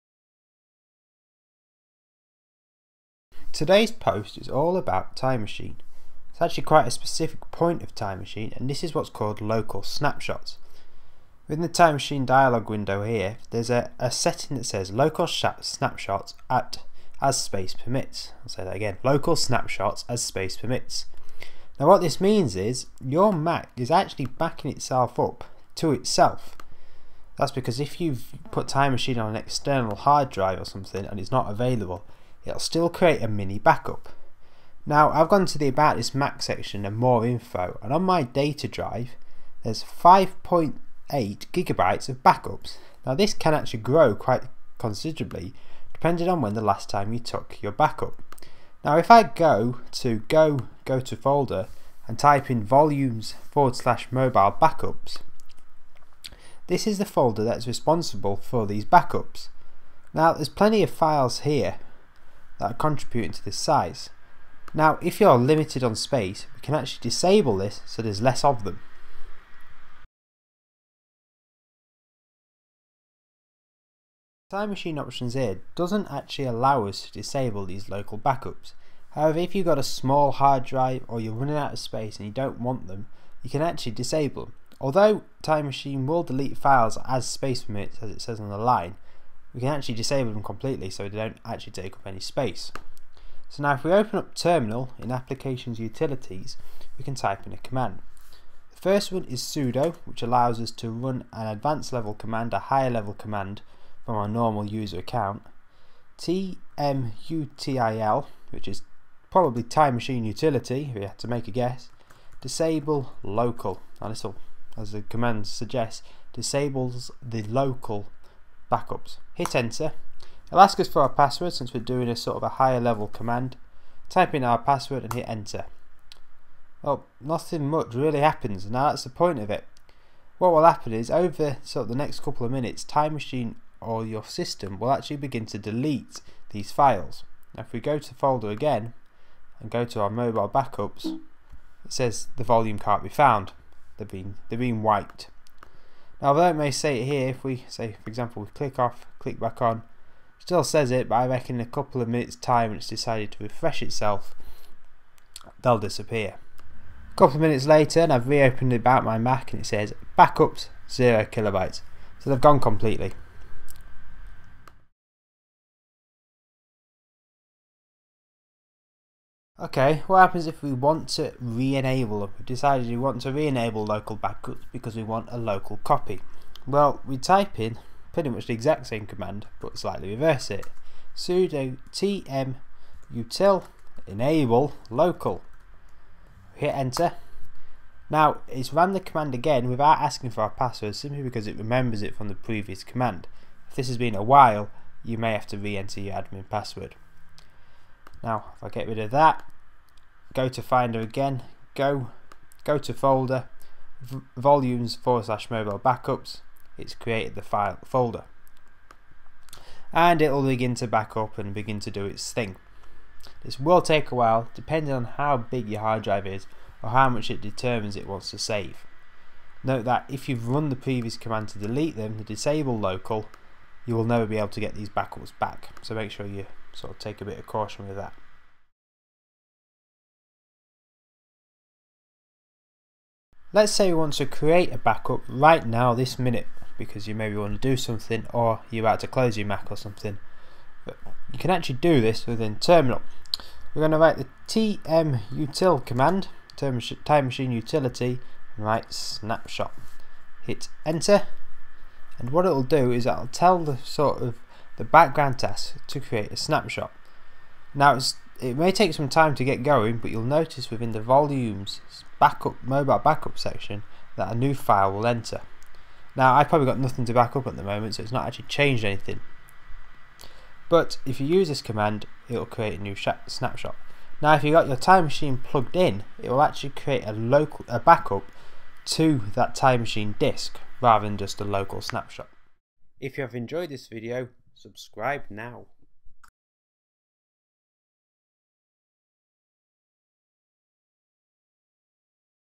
Disabling Local Time Machine Backups On Mac OS X
Your Mac stores a local Time Machine backup on your local disk. You can delete the local time machine backup on your Macintosh HD drive to save some space. This tip also shows you how to re-enable them.

The command in the video is the following:
sudo tmutil disablelocal